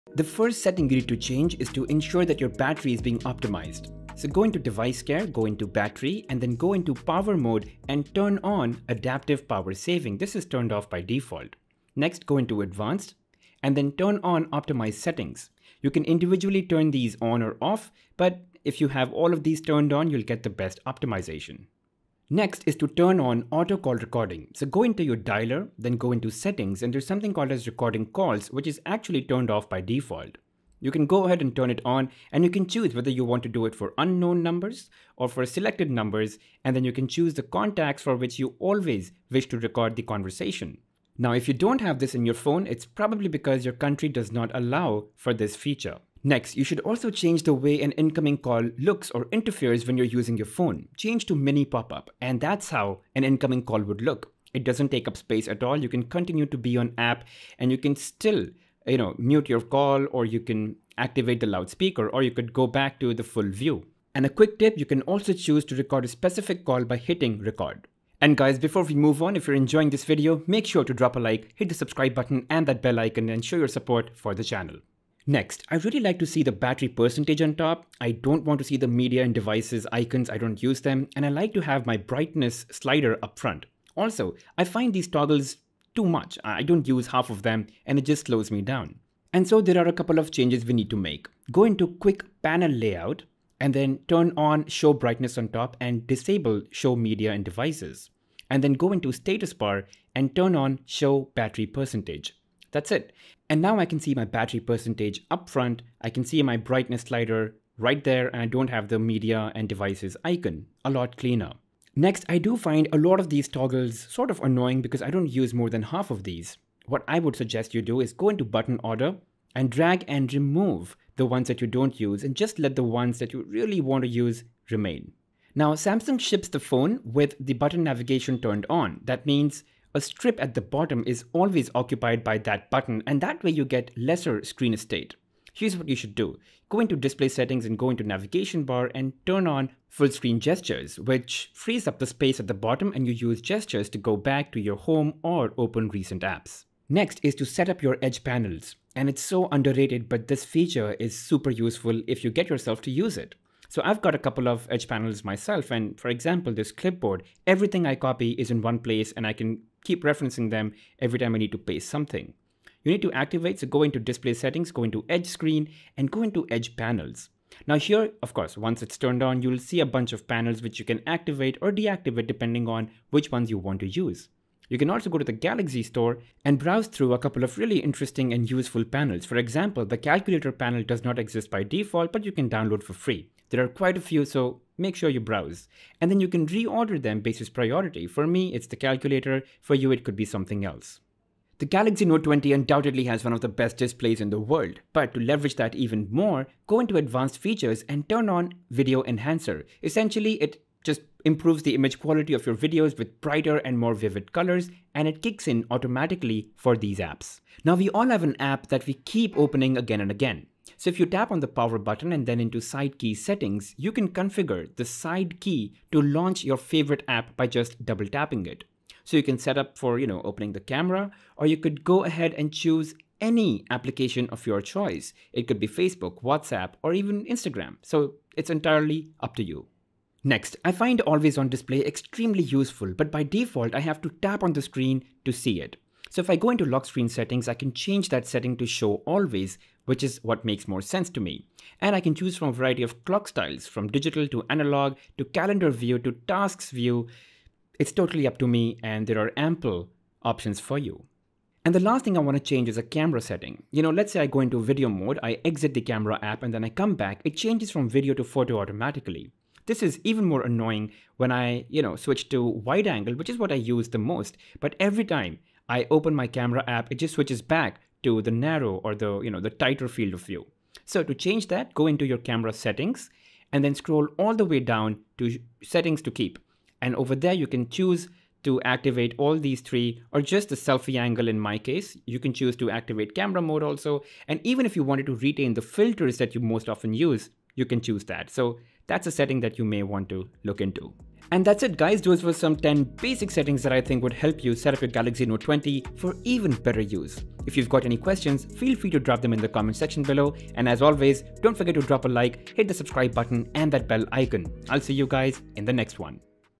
New Galaxy Note 20 (Ultra)? 10 Basic Settings You Must Change NOW!
Just got your new GalaxyNote20 GalaxyNote20Ultra - then you must change these settings right away to get the most out of your phone.

These are One UI 2.5 features and are common across all Galaxy smartphones that run on One UI 2.5 or even One UI 2.1.

Battery Optimization: 00:00
Auto Call Recording: 00:22
Mini call pop up: 00:46
Customize status/notification area: 01:15
Get more display area: 02:05
Setup Edge Panels: 02:27
Video Enhancer: 03:11
Quick Launch Favourite App: 03:27
Show AOD ALWAYS: 03:50
Store Last Camera Setting: 04:11